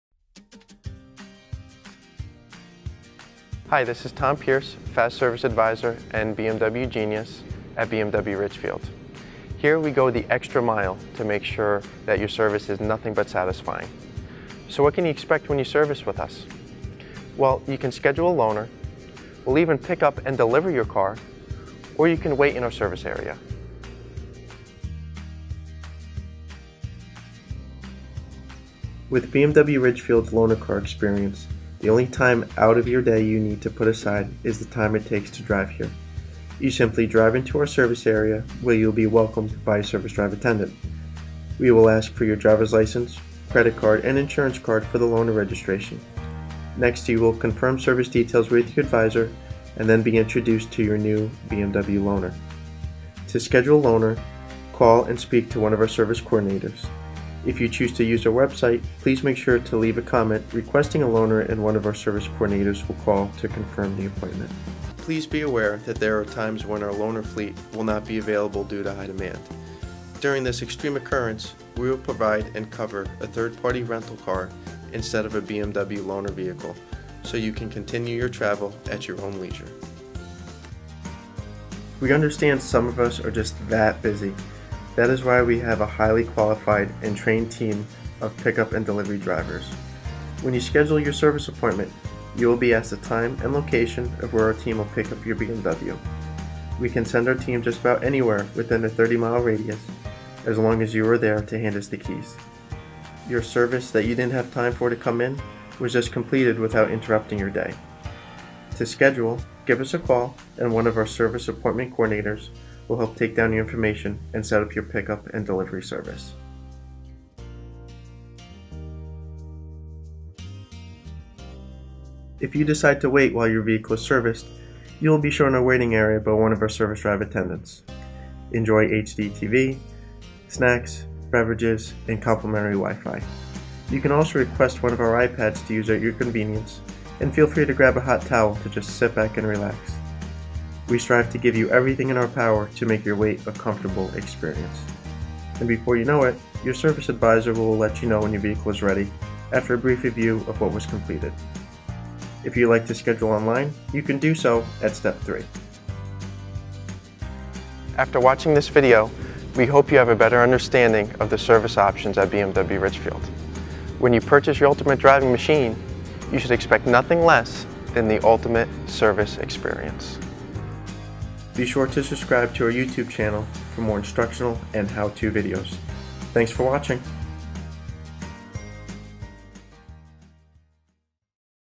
What To Expect In Service
What is pickup and delivery? Do I need to bring anything with me? Will I need a loaner?

Watch Service Advisor Tom answer these questions and more pertaining to your next service visit at BMW of Ridgefield.